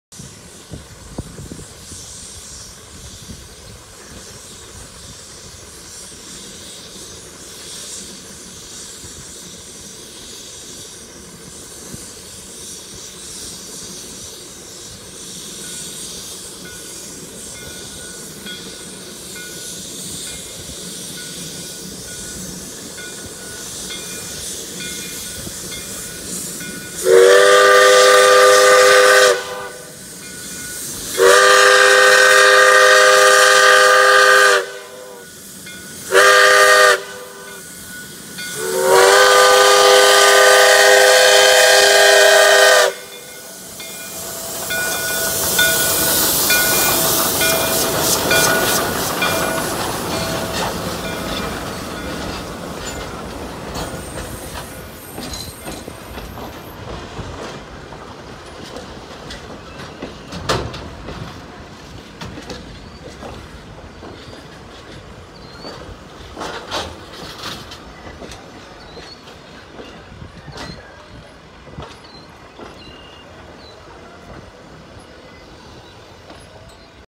J. Neil’s 5 at Illinois Railway Museum
The auxiliary tender behind the shay was on the train for testing purposes only.